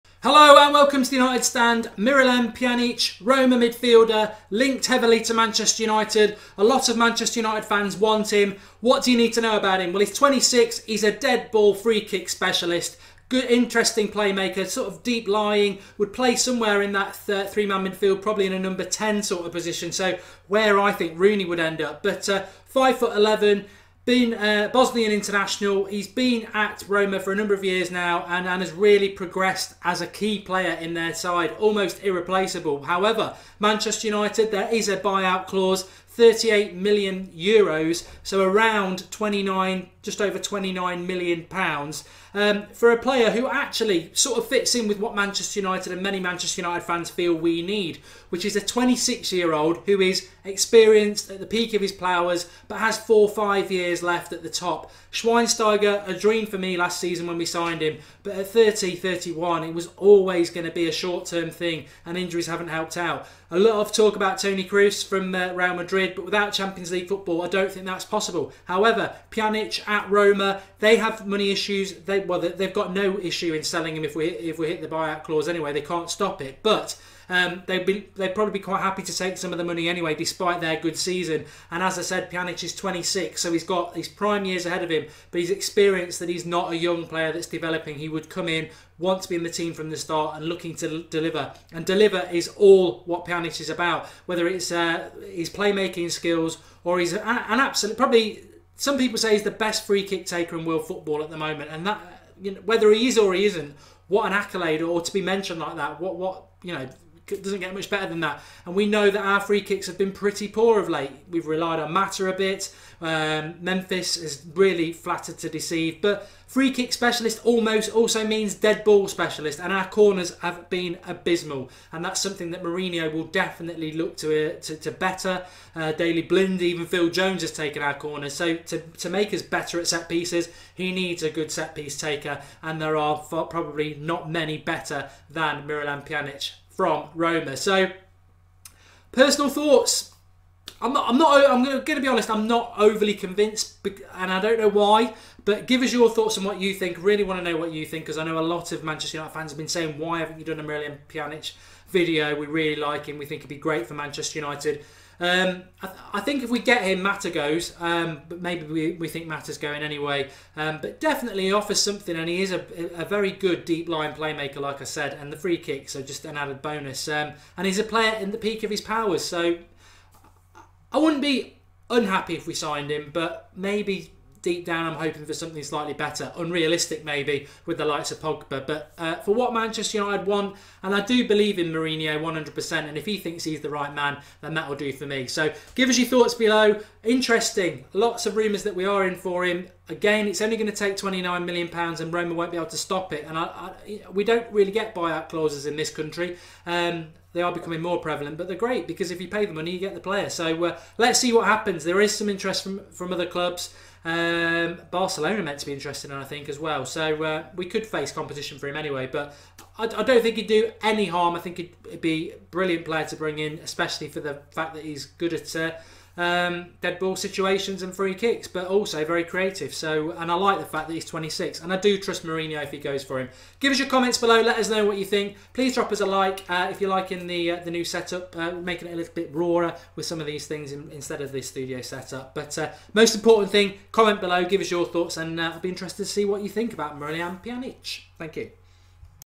Pjanic Manchester United transfer moves closes | MUFC Latest
Miralem Pjanic could be joining Manchester United in a 29 million pound deal from Roma. Watch & give us your thoughts on free kick specialist Pjanic.